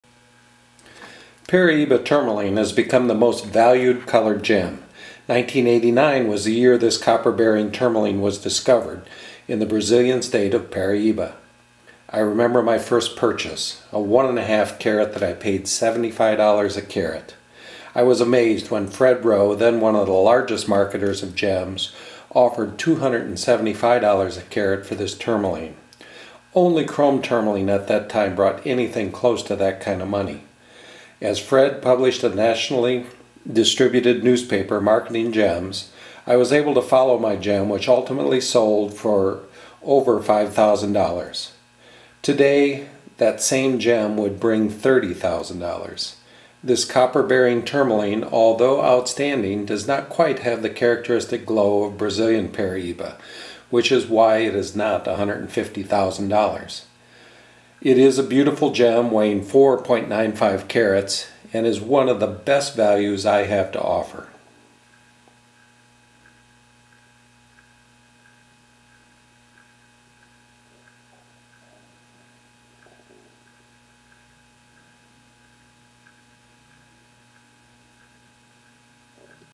Moregems.com Gemstone Paraiba Tourmaline 4.95 ct.
bearing Paraiba tourmaline from Mozambique weighs 4.95ct. beautifully cut in a pearshape by Steve Moriarty.  High clarity and precision cut makes this a rare find.